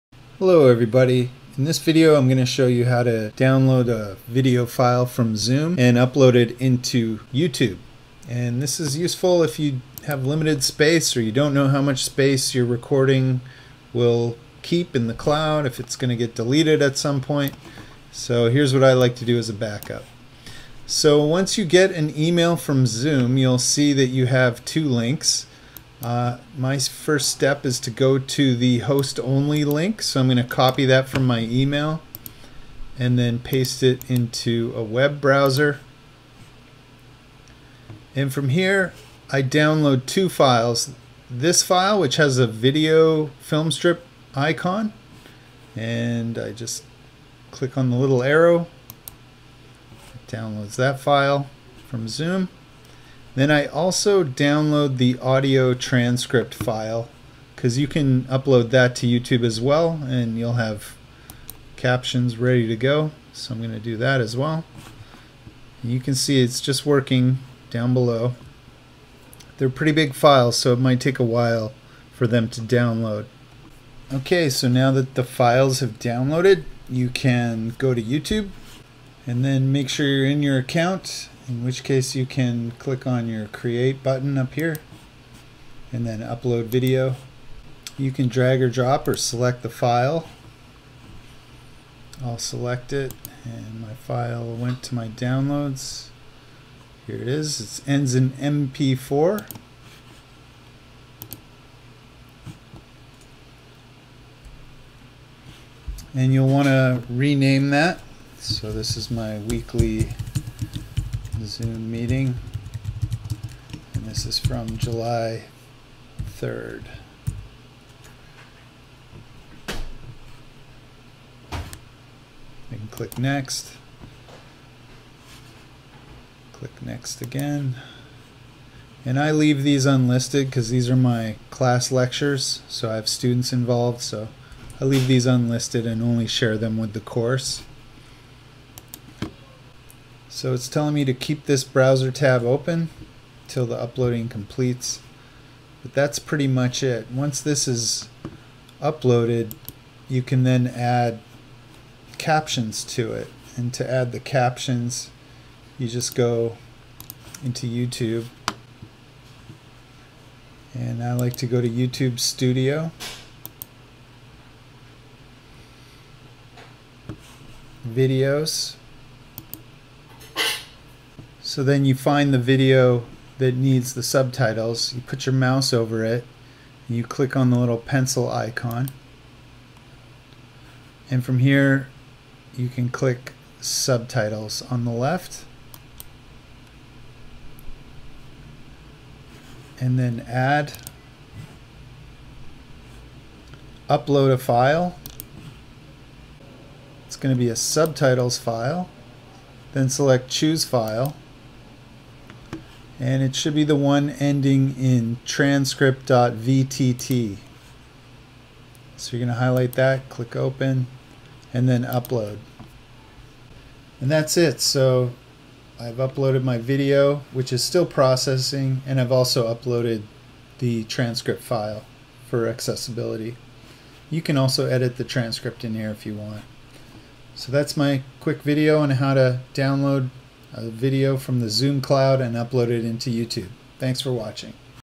How to upload a Zoom Cloud recording with transcripts to YouTube
Depending on your Zoom subscription you may have limited cloud storage. This video shows how to download a video and transcript file from the Zoom cloud and upload to YouTube.